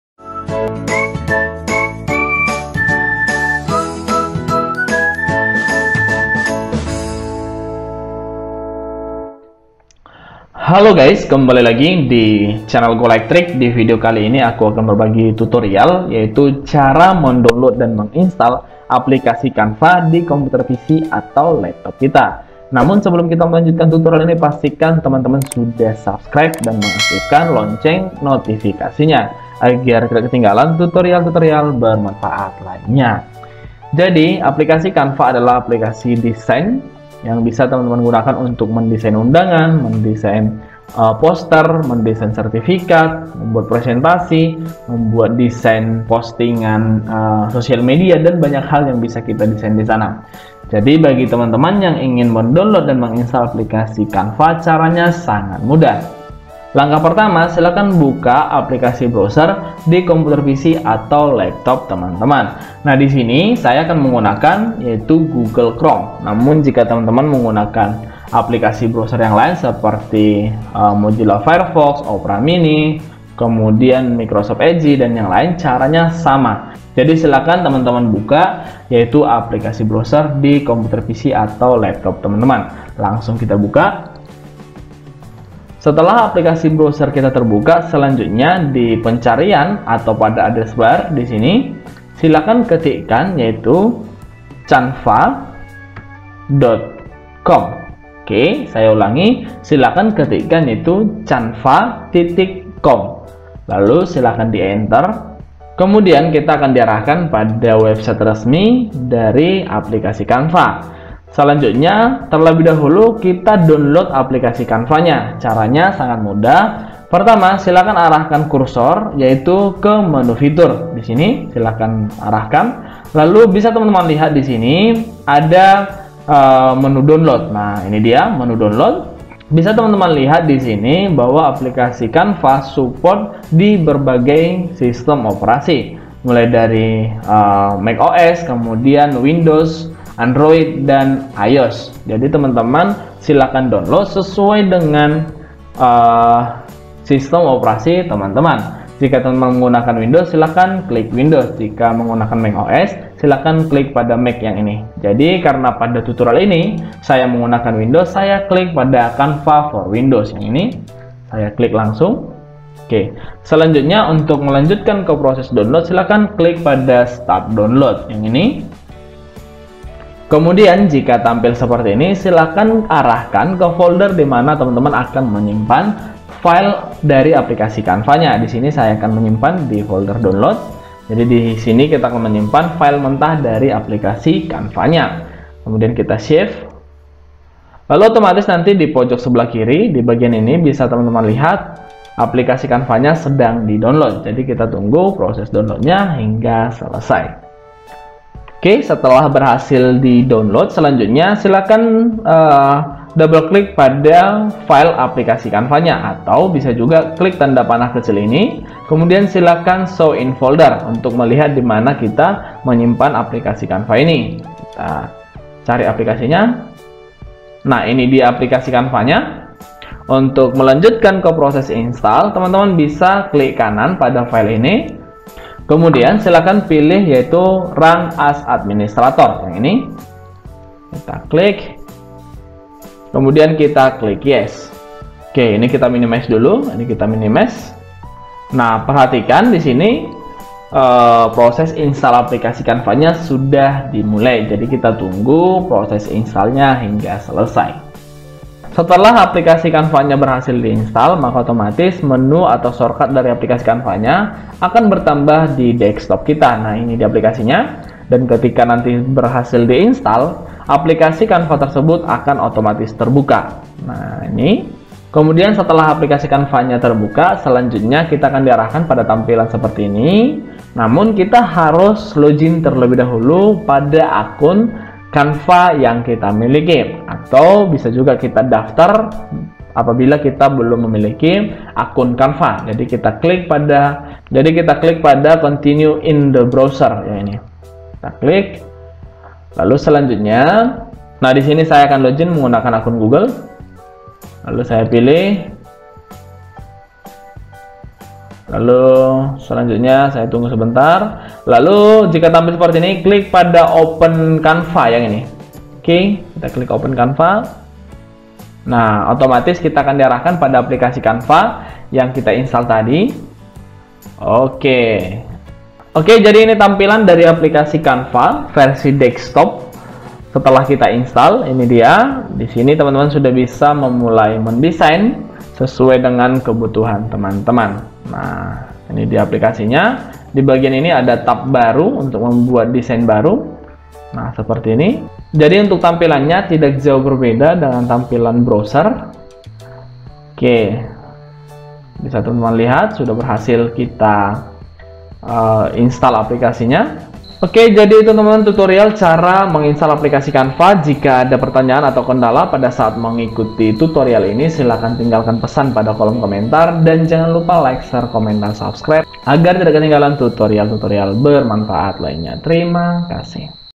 Cara Download dan Install Aplikasi Canva Di Laptop | Install Canva Di Windows
Berikut adalah tutorial cara install aplikasi canva di laptop/pc, dengan tutorial ini kalian bisa mendownload dan menggunakan aplikasi canva di laptop kalian.